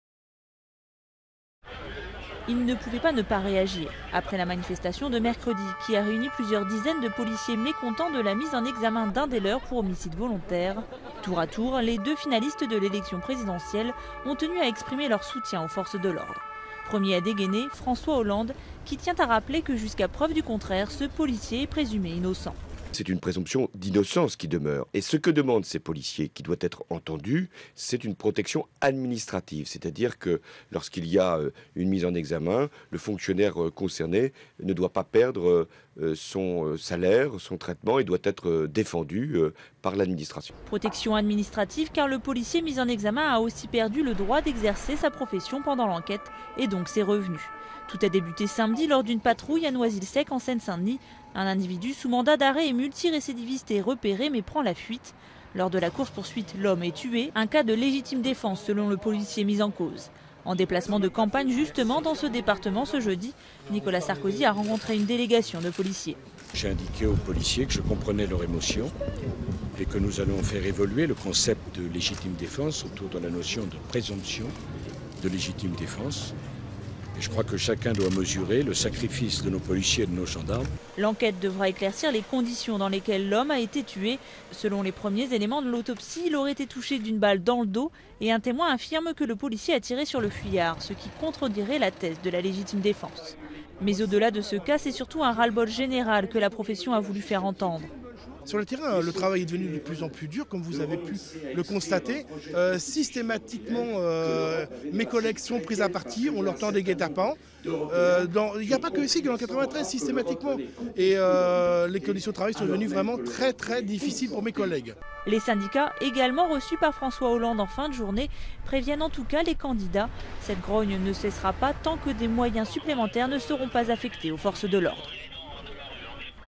Le ras-le-bol des policiers s'invite dans la campagne
Les deux candidats à l'élection présidentielle Nicolas Sarkozy et François Hollande ont exprimé jeudi, à des degrés différents, leur soutien aux policiers en colère après la mise en examen pour homicide volontaire d'un gardien de la paix de Seine-Saint-Denis. Durée: 02:06